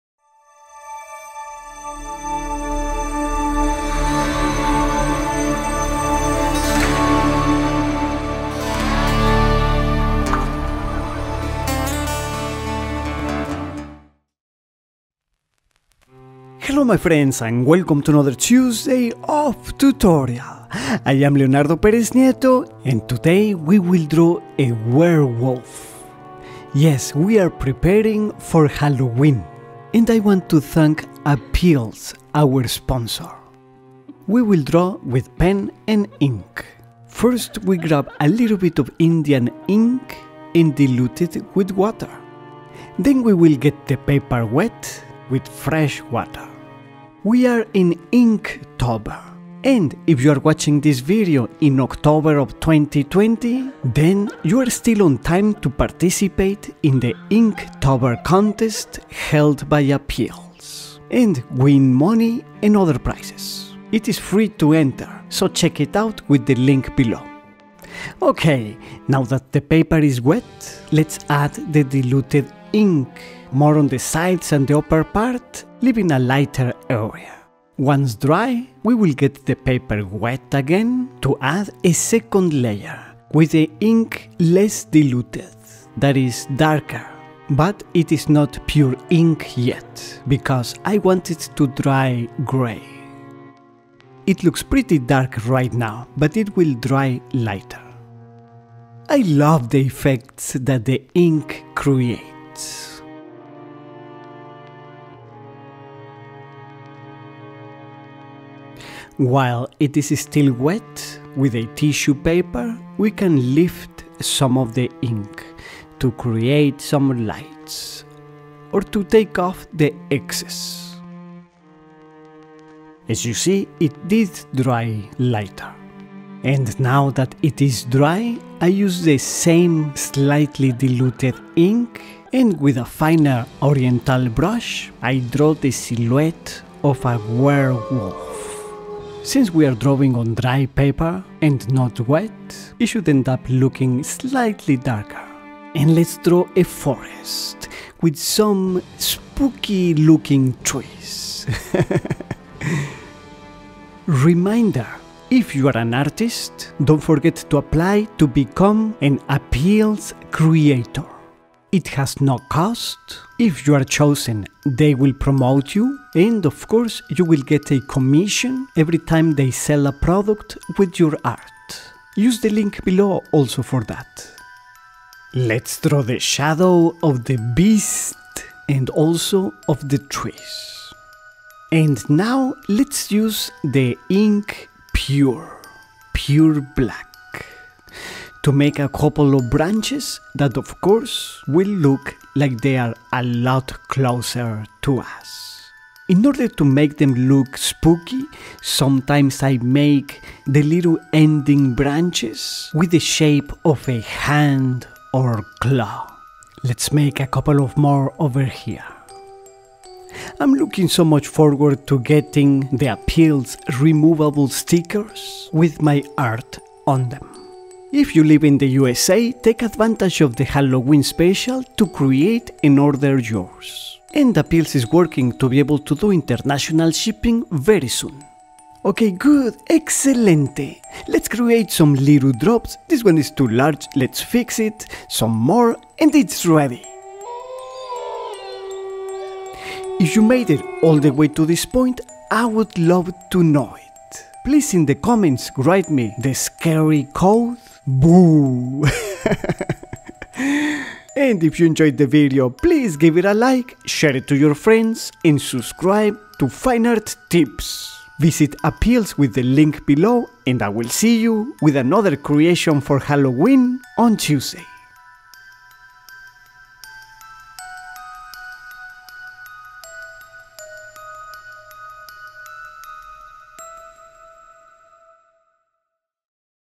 HOW TO DRAW A WEREWOLF WITH INK - for Halloween
Click on this link to participate in the Inktober contest and to visit Apealz
This tutorial shows how to draw a scary werewolf with ink.

List of materials:

- Black India ink (this is not the exact same one that I used, but it is equivalent)

- Large Oriental brush

- Fine Oriental brush

- Fabriano Watercolor Hot Pressed - 9 x 12 inches (24 x 31 cm)